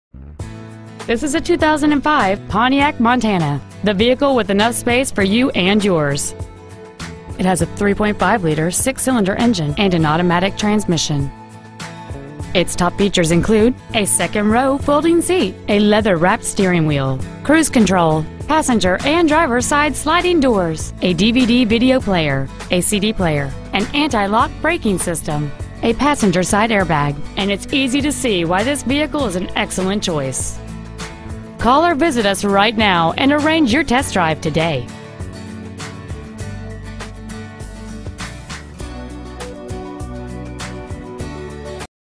Pre-Owned 2005 Pontiac Montana Auburn WA
We are proud to present this 2005 Pontiac Montana.

 Year: 2005
 Make: Pontiac
 Model: Montana
 Engine: 3.5L V6 SFI
 Trans.: Automatic
 Exterior: White
 Miles: 93,875
 Interior: Gray